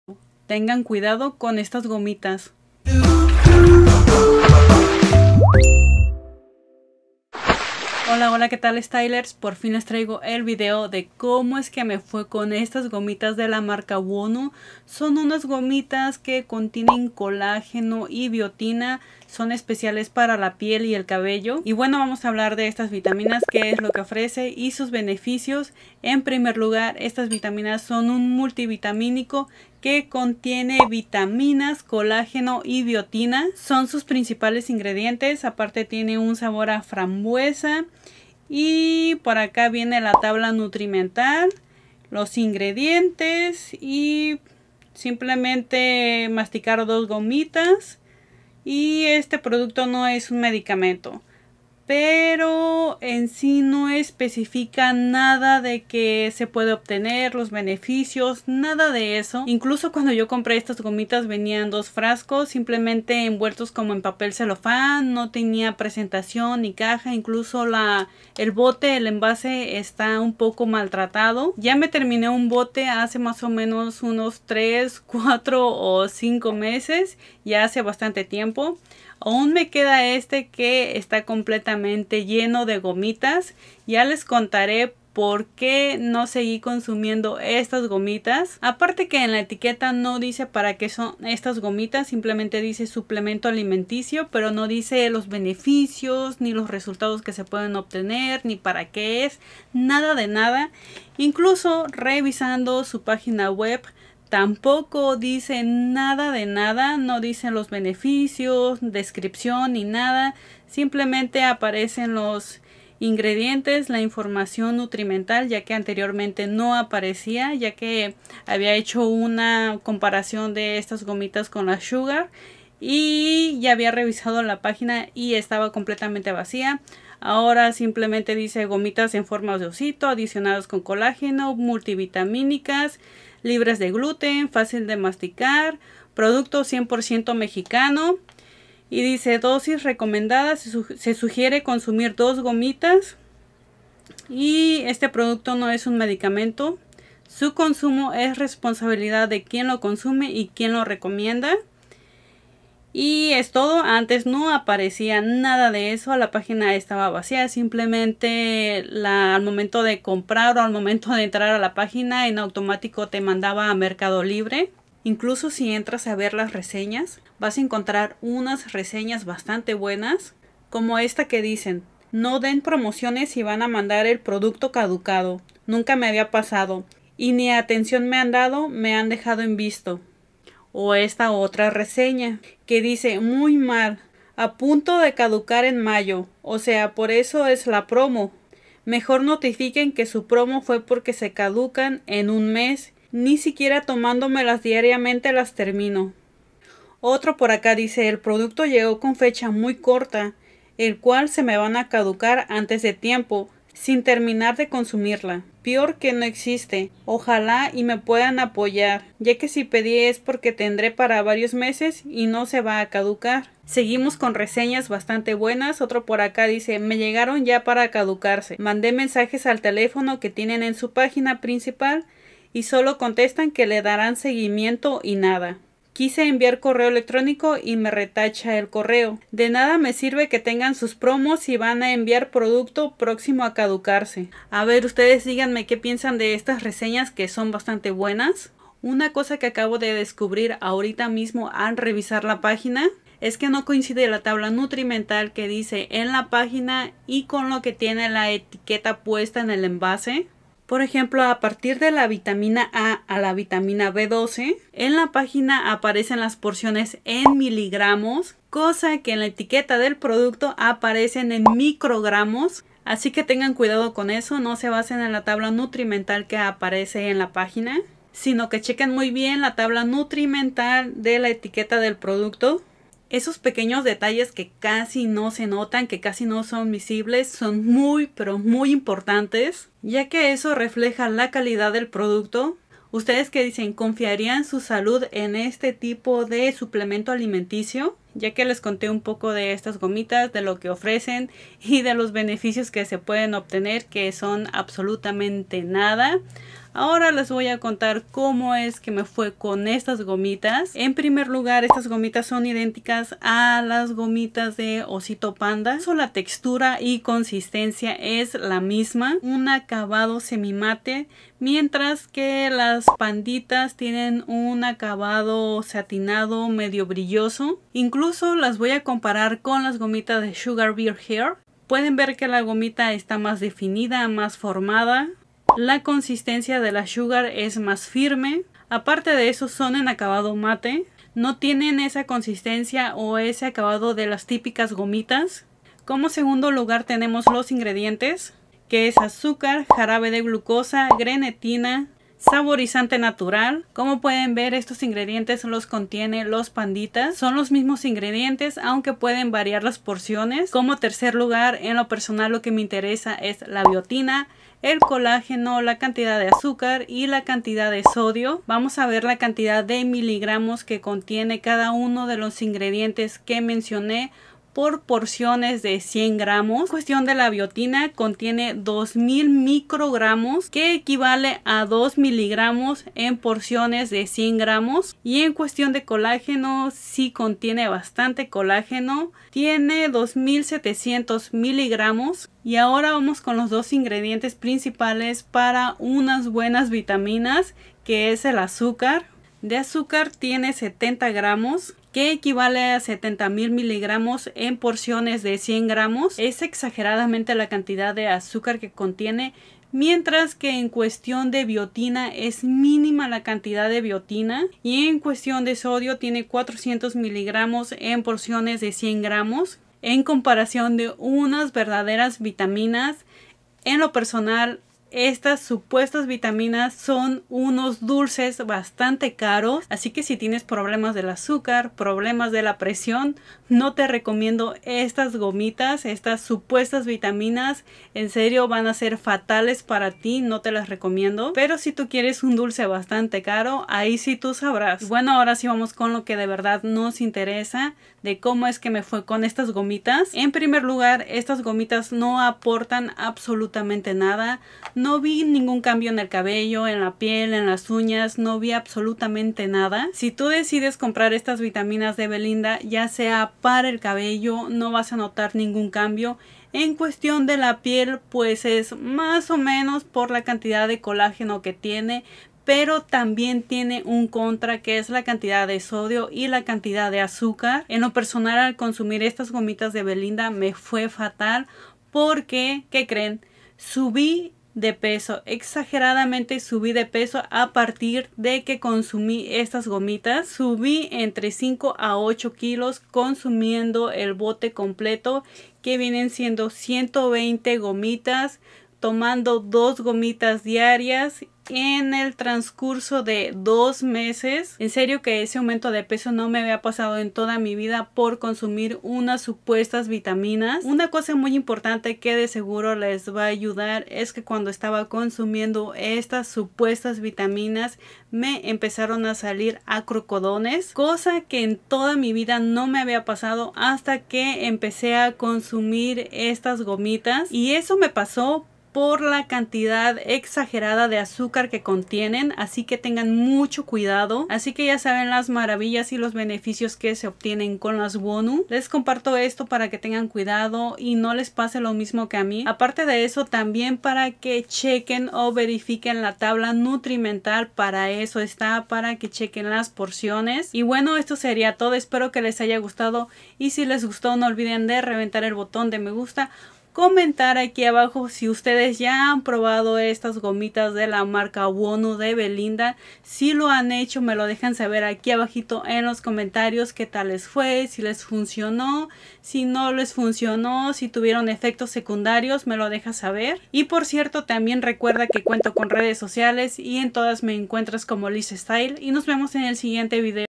CUIDADO!! con estas GOMITAS WONU
Te cuento un poco mi experiencia al consumir la gomitas wonu, para que lo pienses dos veces y tengas cuidado y sobre todo que no te pase lo mismo que a mi..

✴ Code: lizztyle
Halfmoon Alice Gloss Purple

✴Muchas Gracias por Tus Donaciones Me Han Ayudado Mucho a Generar Nuevo Contenido para Ustedes